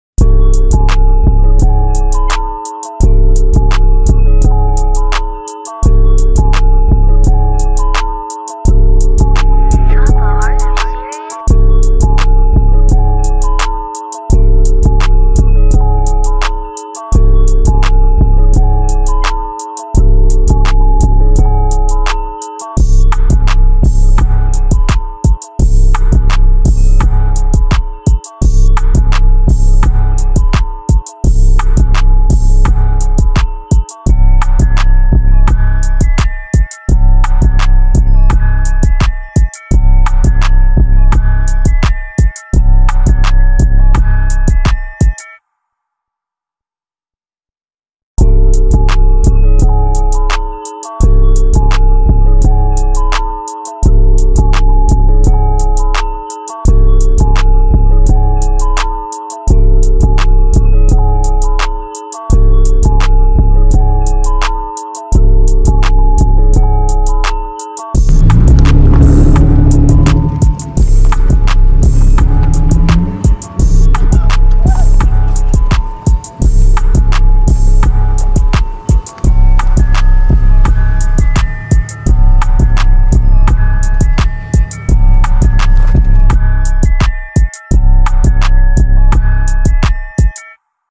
Bagged BMW E46
Quick edit for my friends BMW E46 on bags.